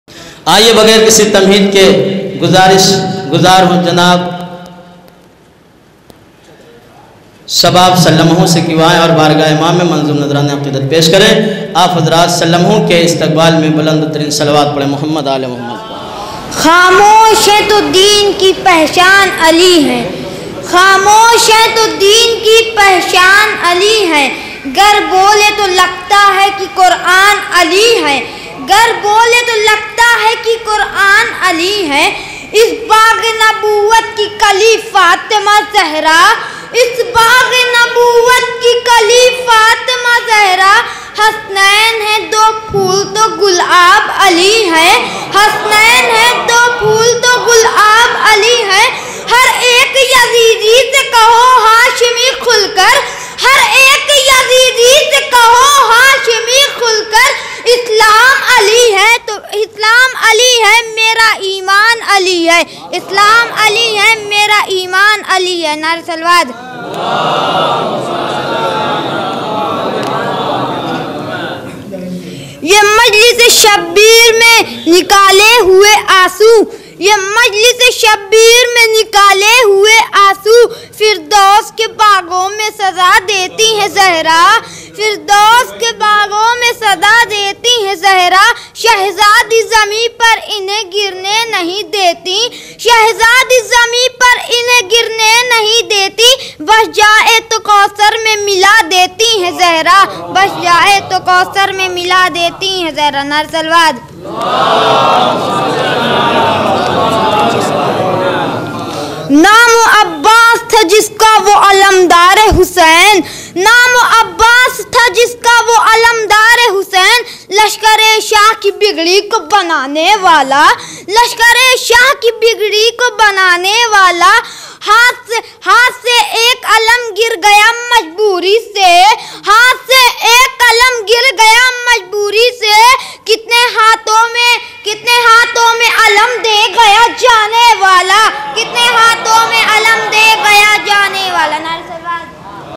Shabab Sallamahu | Majlis e Chehlum | 4 July 2022 | Syeda Jannatul Kubra Marhooma | Rasoolpur
Majlis e Chehlum
Barae Esal e Sawab
Syeda Jannatul Kubra Karbalain Marhooma
Binte
Syed Yaqub Husain Marhoom

Date:
4 July 2022

Bamaqam:
Husainiya Rasoolpur Barwa Saraimeer Azamgarh

Soz Khwani:
Janab Syed Mohammad Sadiq Sahab Rasool Pur Azamgarh

Khetabat:
Maulana Syed Gulzar Husain Jafri Sahab Qibla Taragarh Ajmer

Sogwaran:
Syed Musi Raza Karbalai, Baradaran, Pesran, Duktaran O Ahle Khandan _____________________ ____________________